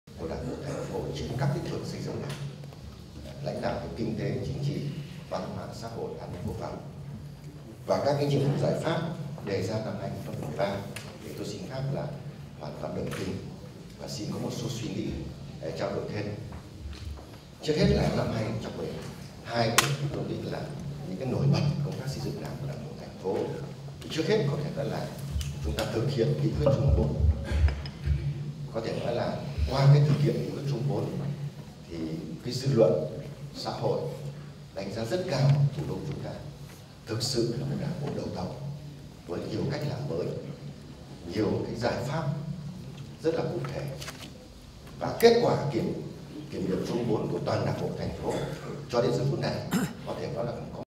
Phát biểu tại Hội nghị lần thứ 11 BCH Đảng bộ TP Hà Nội khóa XV